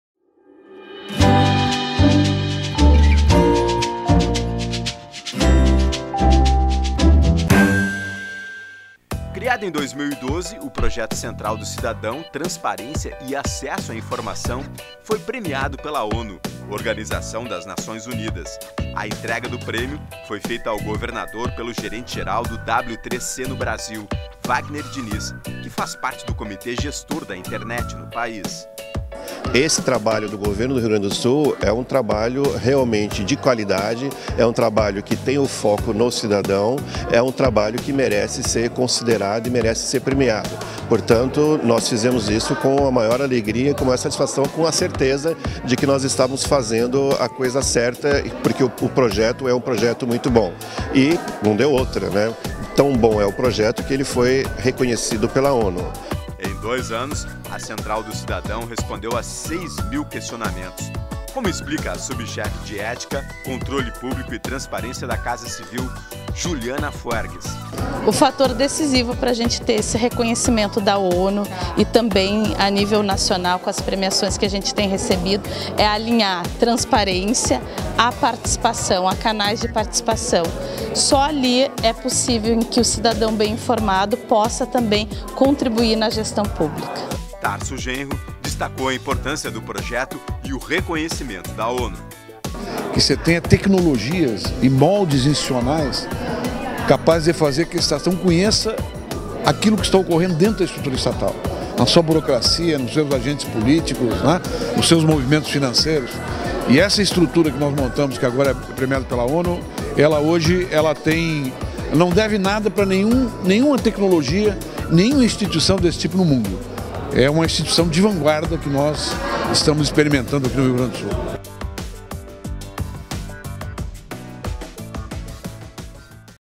Central do Cidadão entrega Prêmio da ONU ao Governo do Estado
Participam do ato no Piratini o governador Tarso Genro, o coordenador do W3C Brasil, Wagner Diniz, a representante da Unisinos, Susana Kakuto, o coordenador do CEGOV/UFRGS, Marcos Cepik e os gestores locais da Lei de Acesso à Informação.
A Central do Cidadão é o portal criado pelo governo gaúcho para atender as determinações da Lei de Acesso à Informação. Criado em 2012, o Serviço de Informação ao Cidadão está em constante atualização e, atualmente, conta com serviços de transparência ativa e a solicitação de informações, canais de Ouvidoria e Denúncias, banco de dados abertos, além de ferramentas como o Mapa da Transparência e a Cartilha da Transparência.